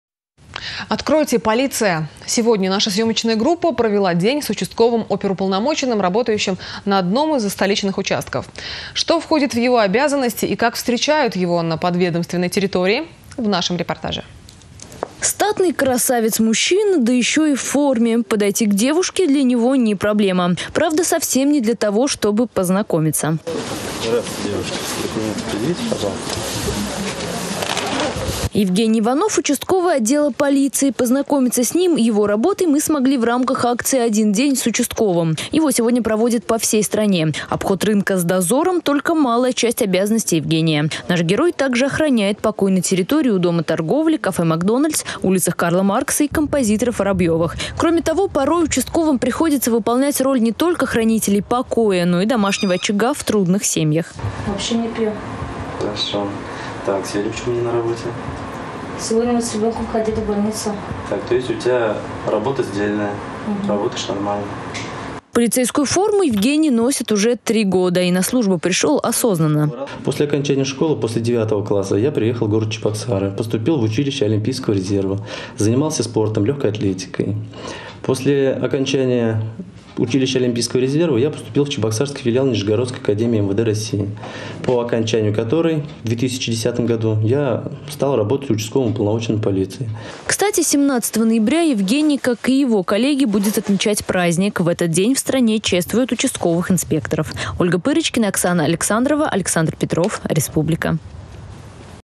Откройте, полиция!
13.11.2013
Наша съемочная группа провела день с участковым оперуполномоченным. Что входит в его обязанности и как его встречали на «земле» - в репортаже.